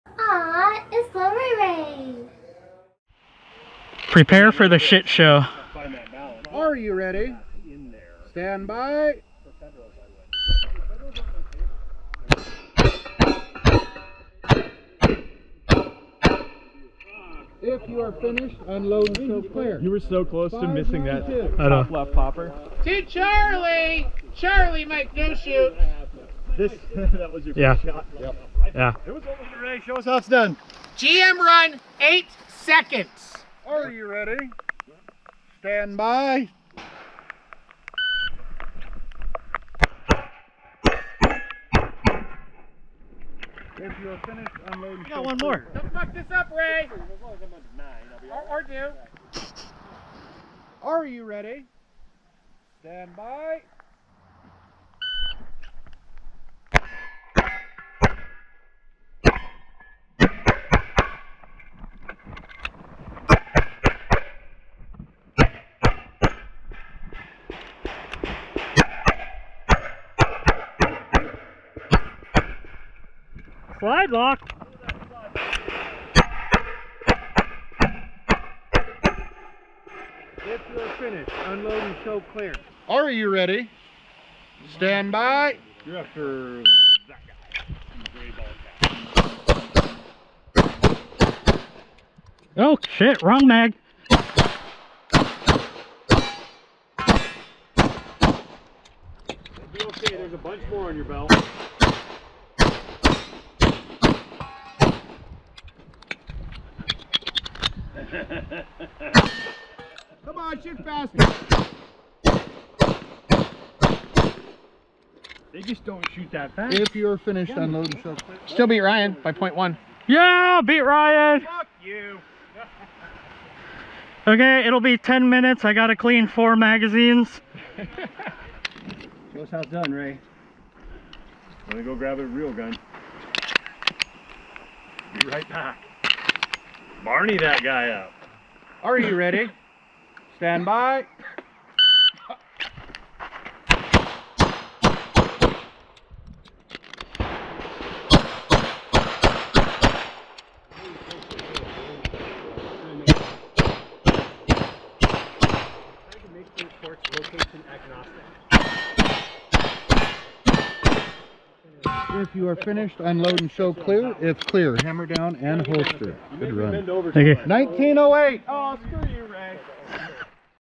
3 July 2022 - Custer USPSA
23rd Overall out of 40
3rd Singlestack out of 9
82.17%

Intro provided by the amazing Britt D.